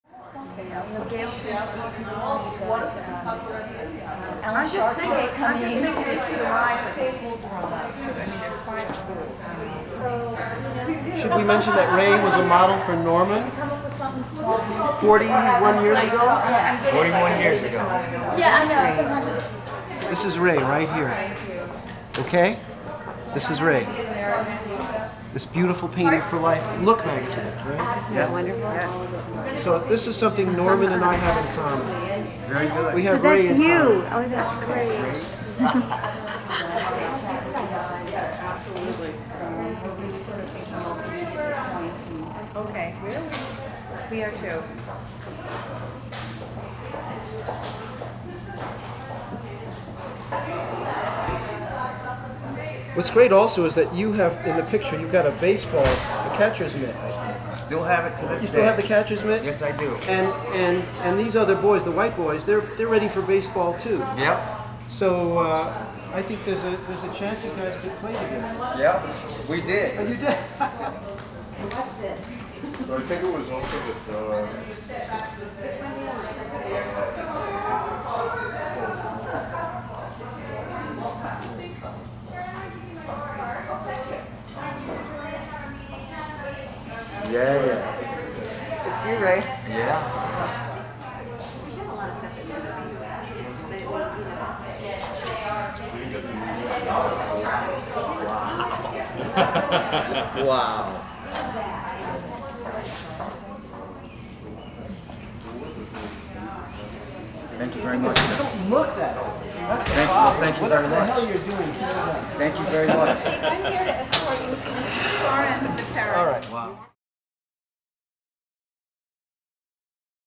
Steve Brodner Meets "The New Kid in The Neighborhood"
Illustrator Steve Brodner took a break from politics during a recent visit to Norman Rockwell Museum, to draw a caricature of Wray Gunn, the original model for Norman Rockwell's 1967 painting "New Kids in The Neighborhood."

"Raw Nerve! The Political Art of Steve Brodner" is on view at Norman Rockwell Museum through October 26, 2008.